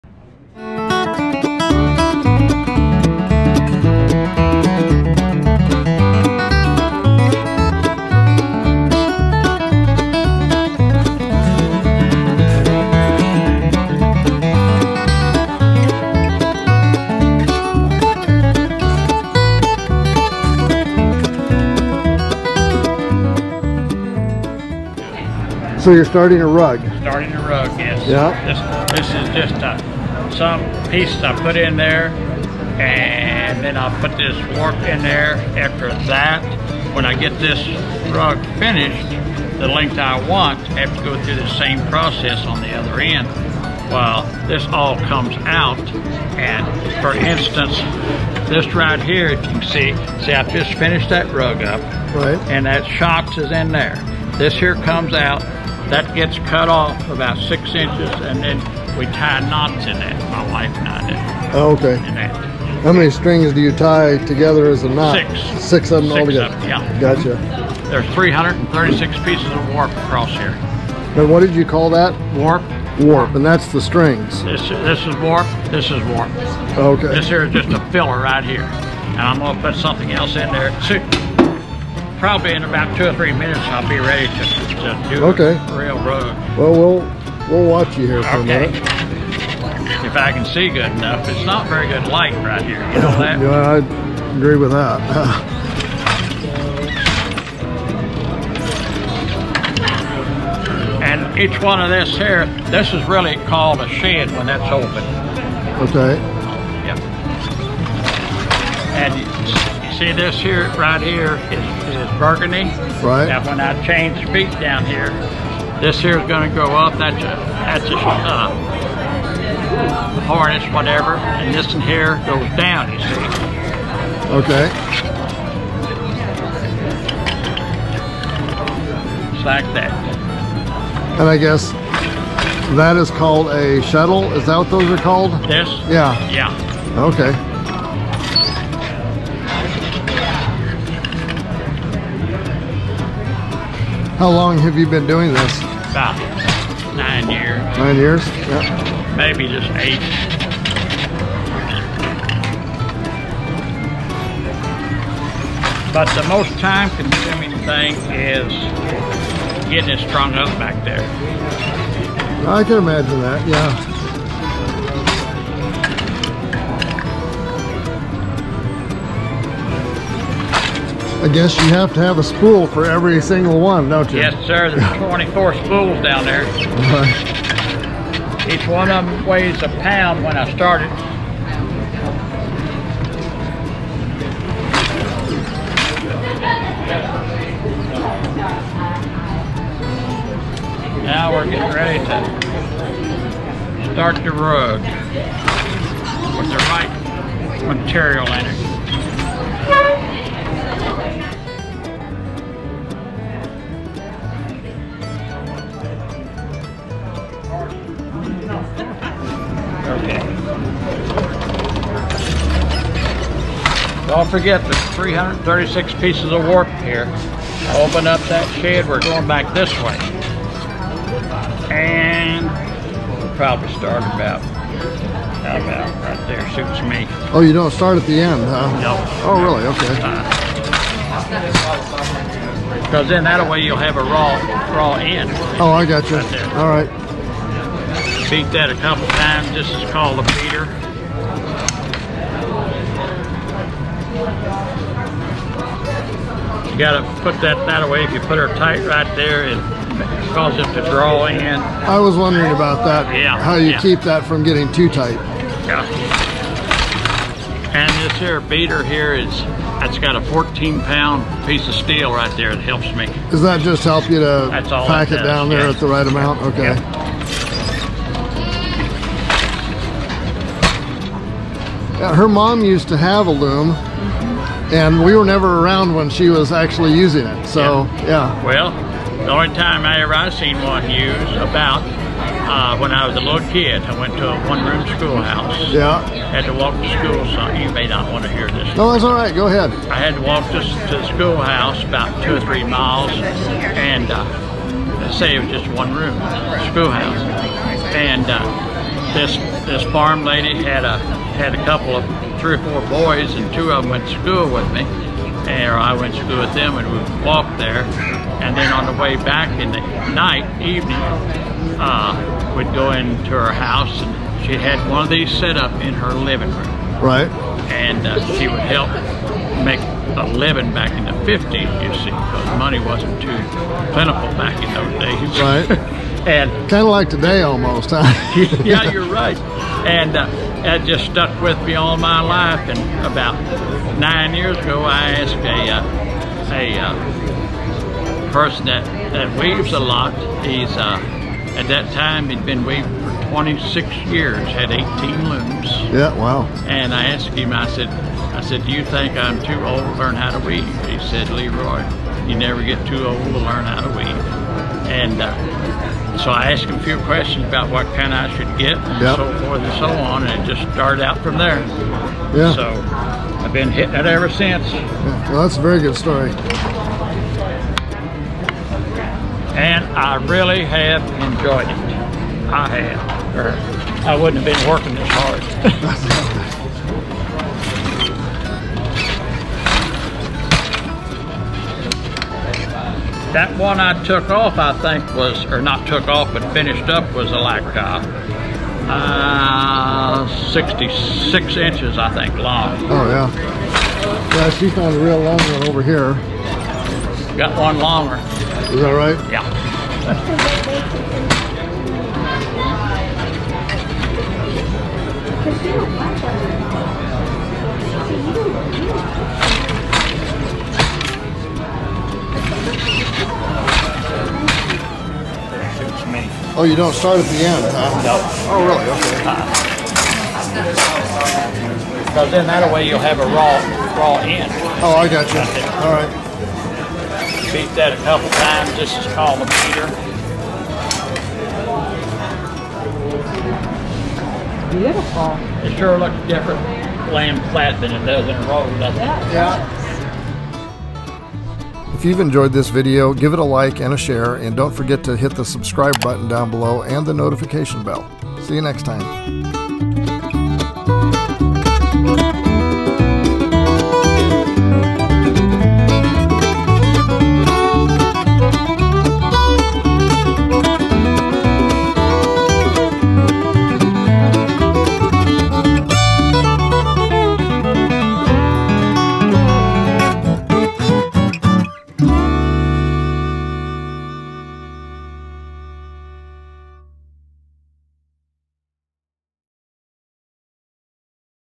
Mountain Craft Rug Weaving
Another in our "Mountain Craft" playlist. The rug weaver in this video even tells a story about the first person he knew that did weaving, did so to earn extra income from it back in the 50's. It's still a viable source of extra income or barter.